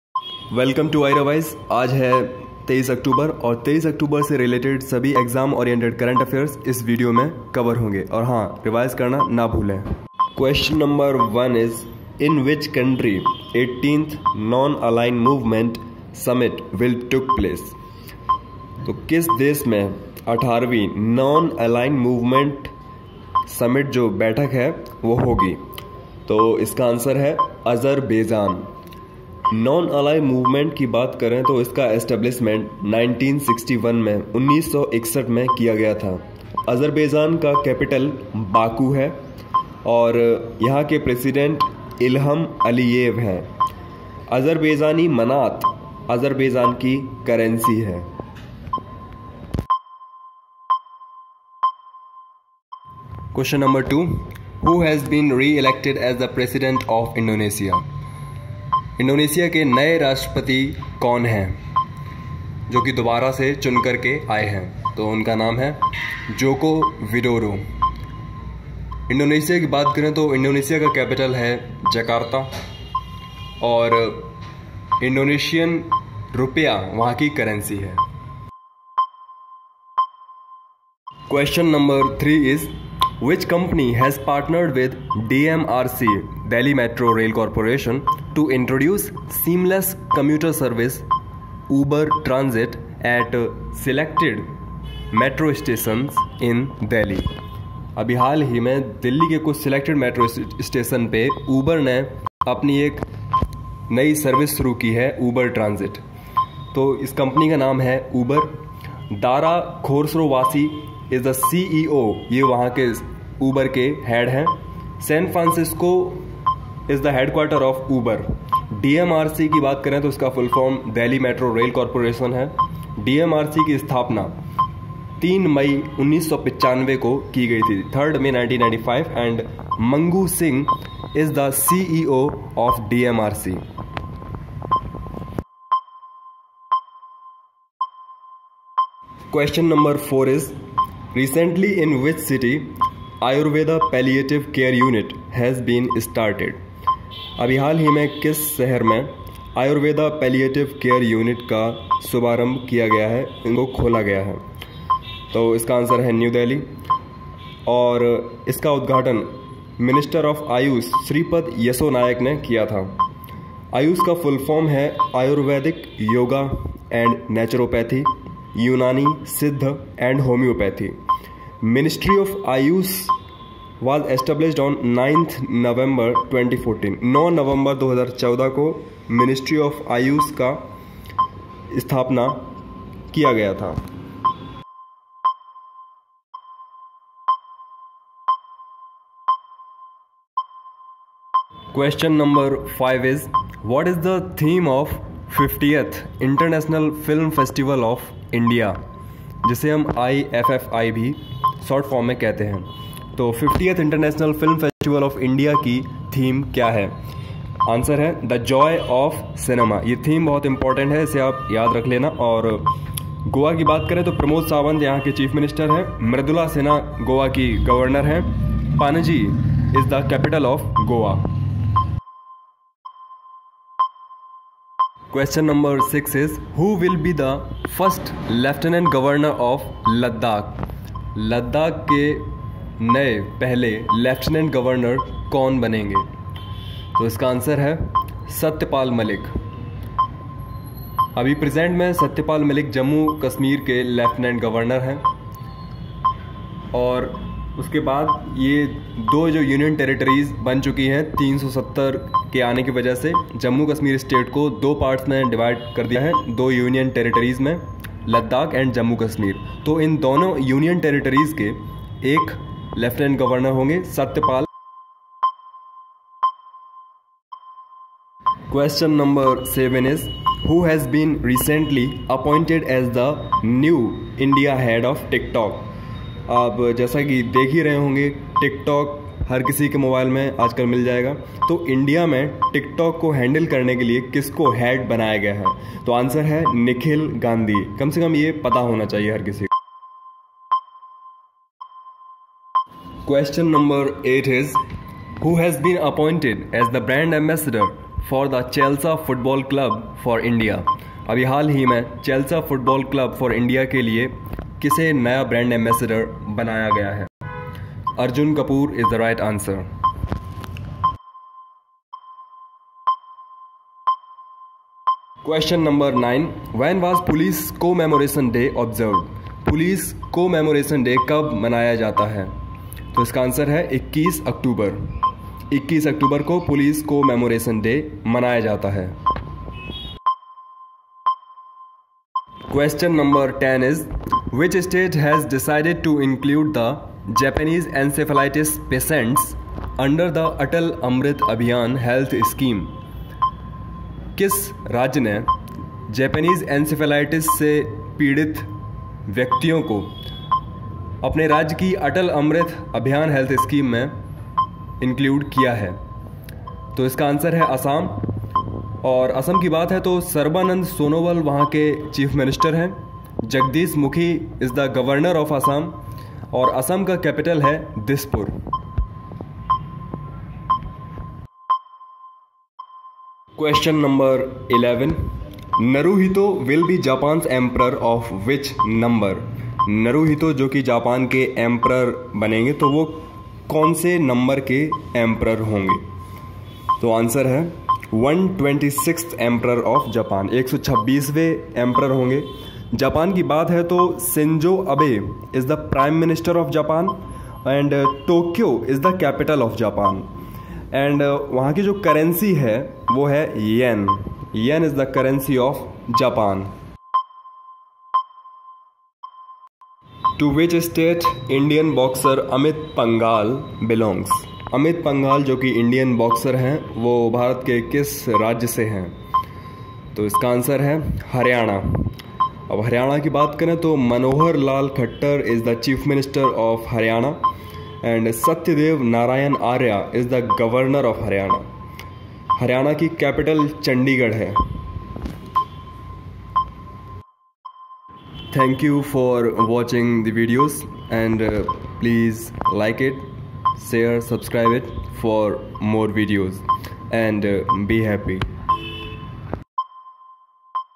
i Revise #4 | 23 october 2019 current affairs | daily current affairs | SSC | BANKING | RAILWAY |CPO
23 OCTOBER 2019 current affairs.
This is the second video of current affairs uploaded by i Revise, here you will get the most important current affairs related to your exam,

and these current affairs videos are very helpful and important for exams like SSC, BANKING, RAILWAY, CPO, CHSL, Stenographer, and various other exams.

thanks